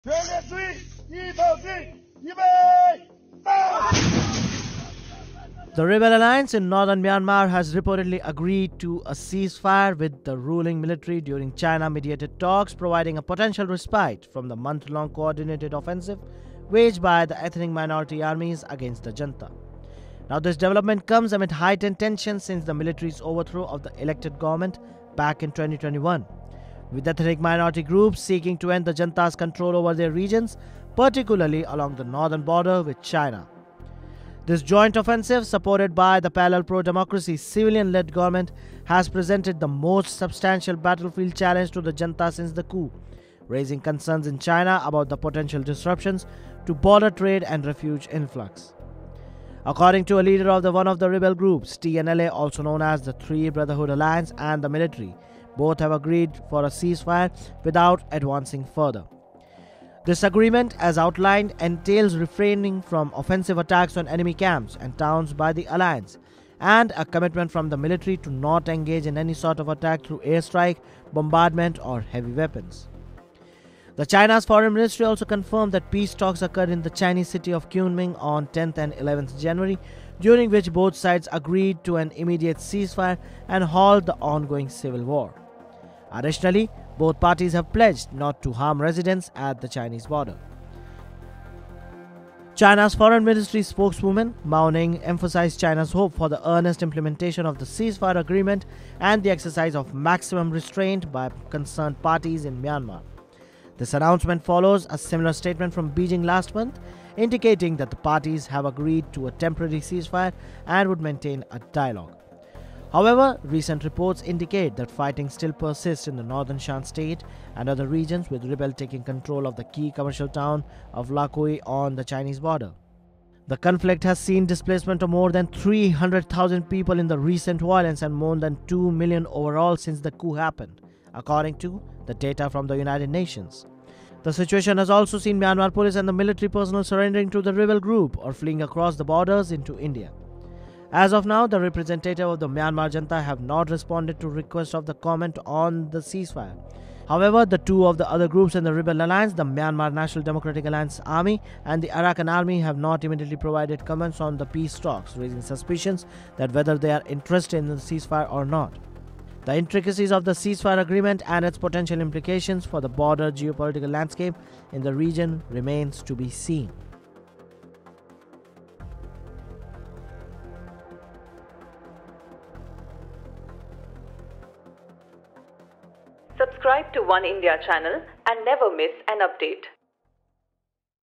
Ceasefire Agreement Reached Between Rebel Alliance and Myanmar Military | Oneindia News
In northern Myanmar, an ethnic rebel alliance has reportedly agreed to a ceasefire with the ruling military during China-mediated talks, offering a potential respite from the months-long coordinated offensive. The agreement comes amid heightened tensions since the 2021 coup, presenting a significant challenge to the junta. Ceasefire details include refraining from offensive attacks and military commitments. China confirmed peace talks and emphasized hope for earnest implementation. The conflict has displaced over 300,000, with implications for regional geopolitics.